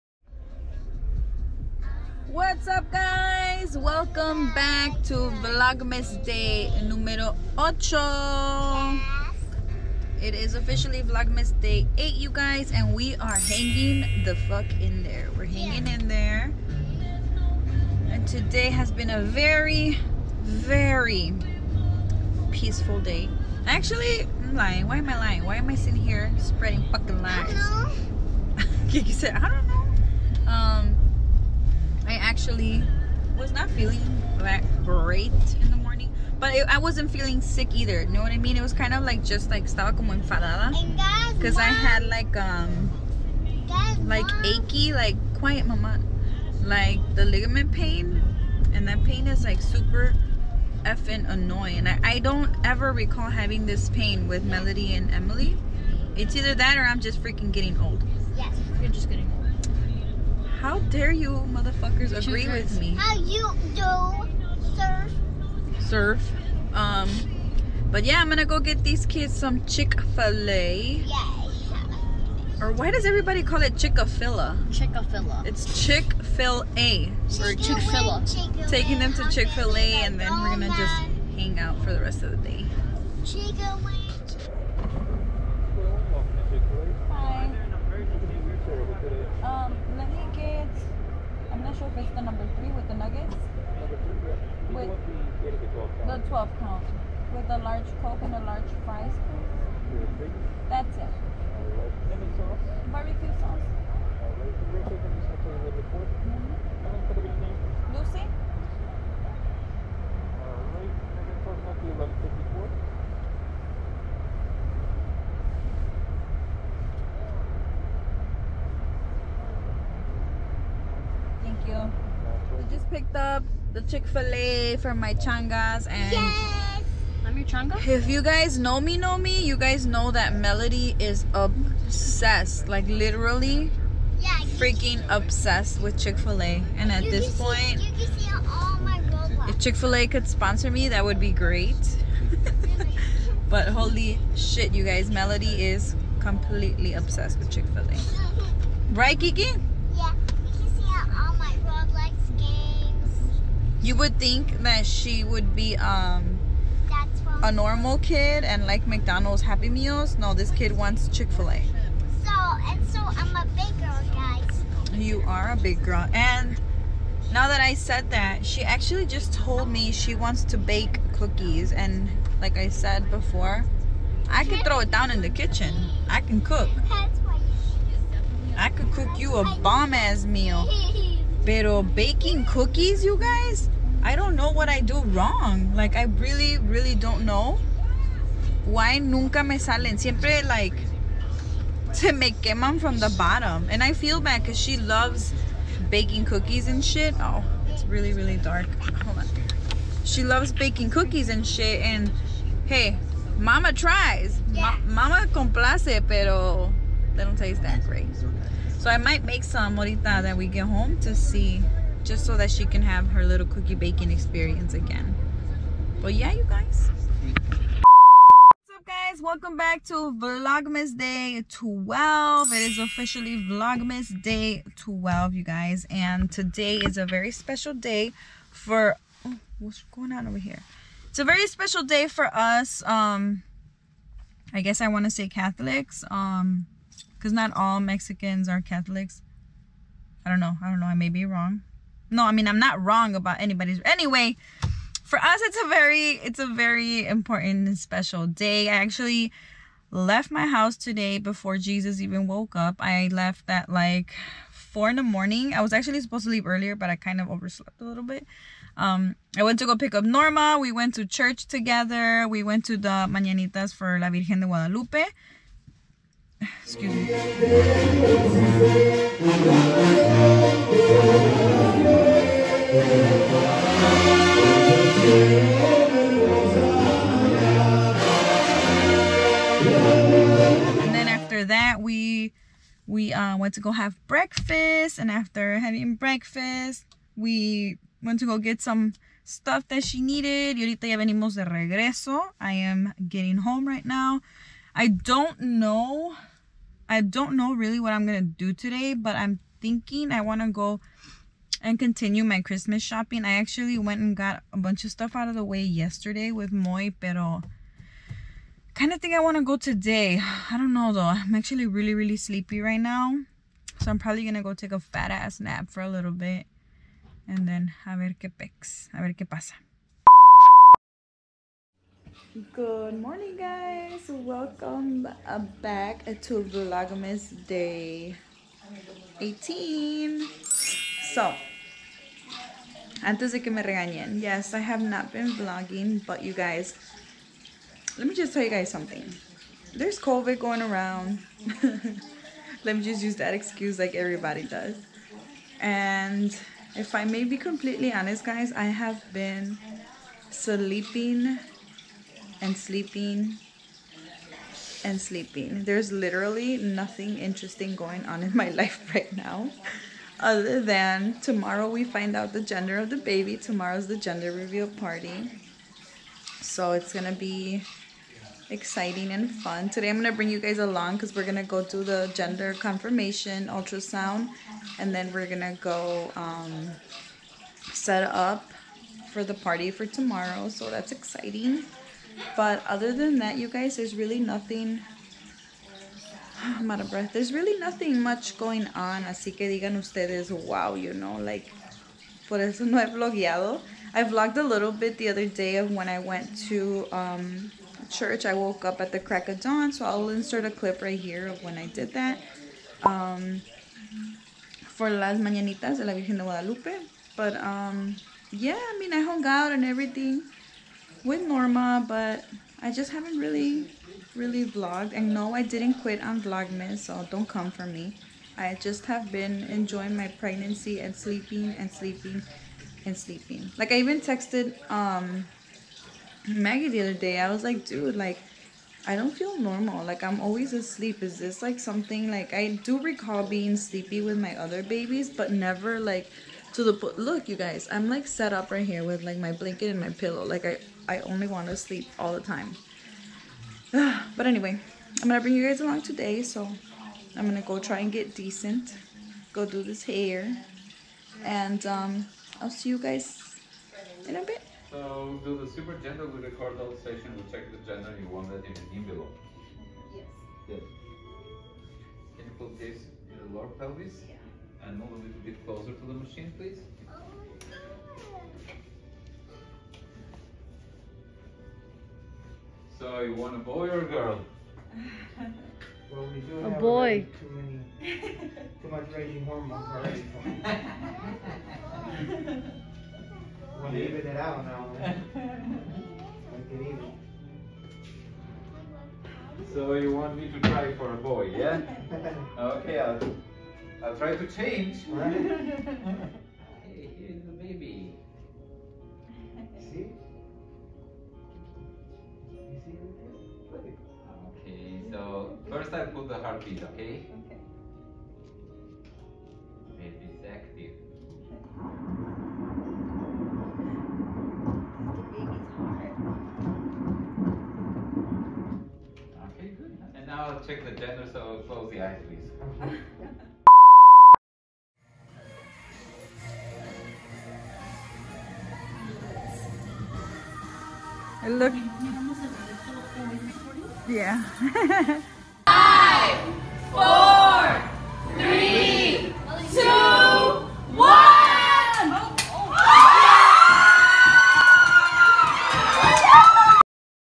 First Peek Gender Confirmation Ultrasound!
We have our results from “sneek peek” blood test however, we wanted to be extra extra sure so we chose to get a gender confirmation ultrasound done at “FIRST PEEK”! It was such an amazing experience! I def recommend this if you want to be super super sure.

LuceroDdak